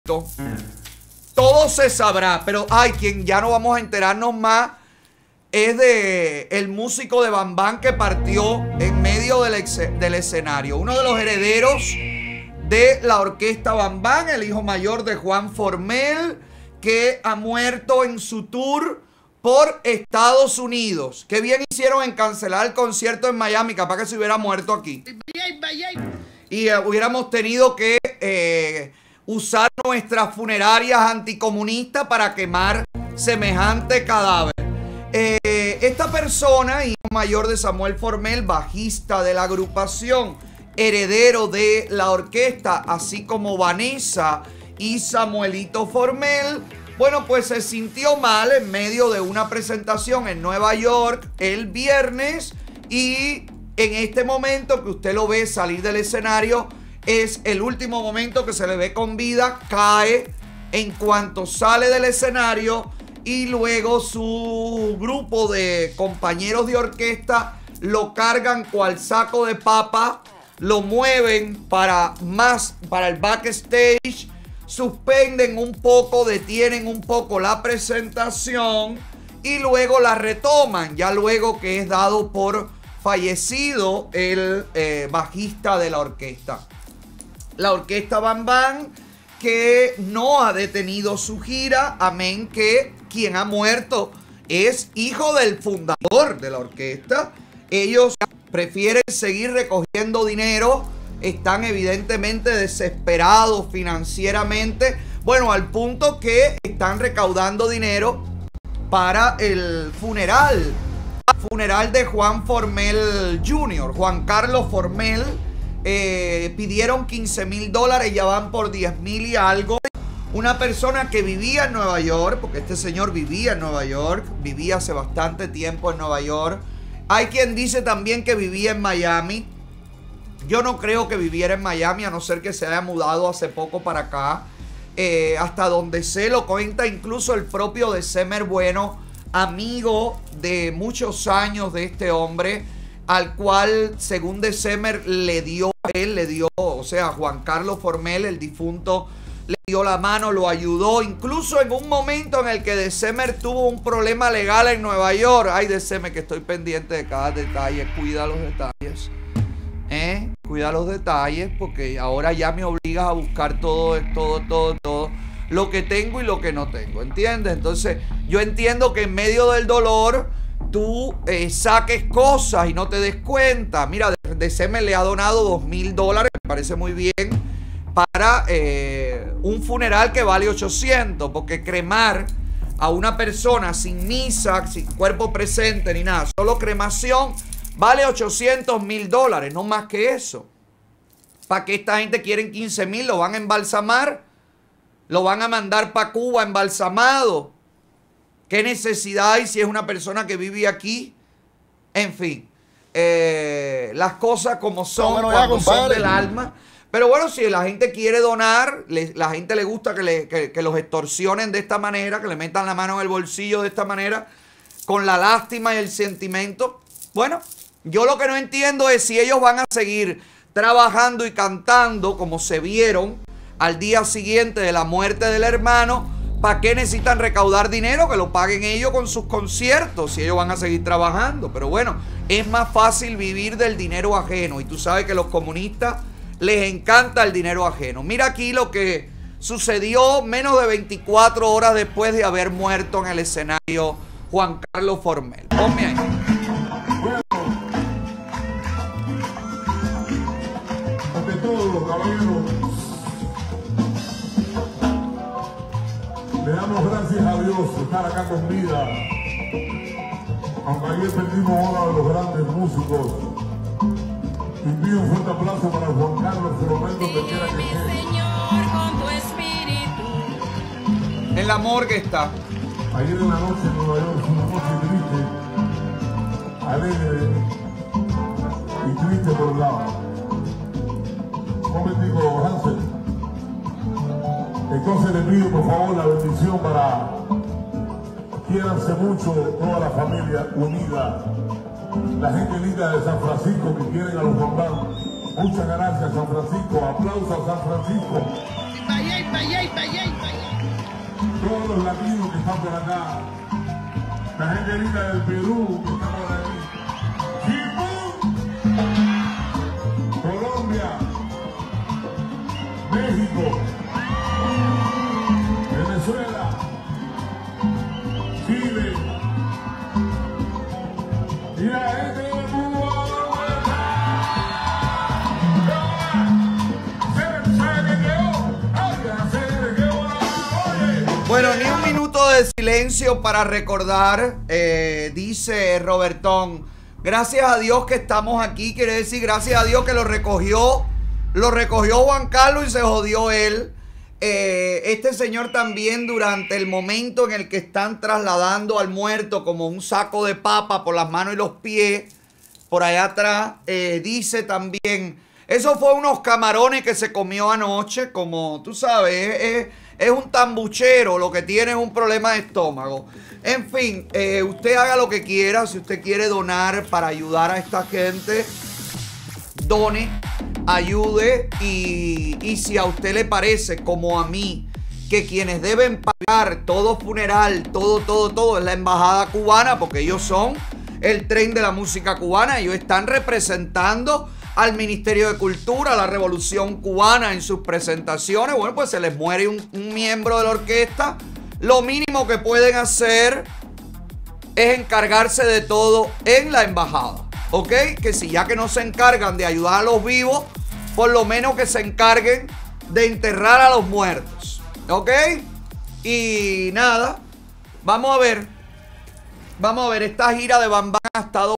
Otaola cuestiona la cifra a recaudar para el funeral del músico de la orquesta procastrista Van Van.
Otaola comenta la repentina muerte de Juan Carlos Formell, durante un concierto en Nueva York y cuestiona la cifra a recaudar para el funeral del músico de la orquesta procastrista Van Van.